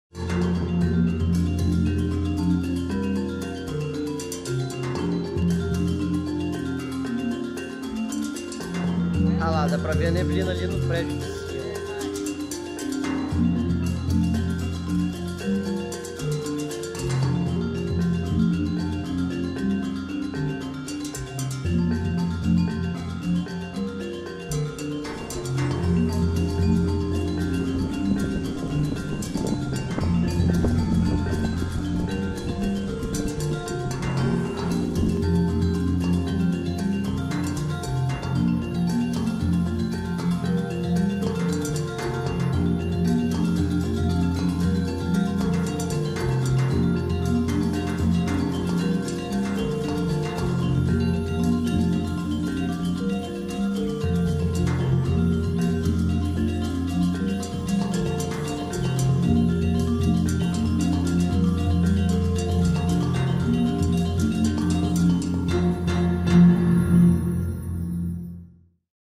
Nevoeiro intenso encobre Vila Velha na chegada da frente fria ao Espírito Santo
Incaper afirma que nevoeiro visto em Vila Velha, neste domingo (30), não passou de um fenômeno natural. Porém, muitas pessoas acreditaram que o fenômeno pudesse ter alguma ligação com os efeitos da erupção do vulcão Tonga. O fenômeno foi provocado pela formação de uma camada de inversão térmica que aprisiona muita umidade na atmosfera mais próxima ao solo.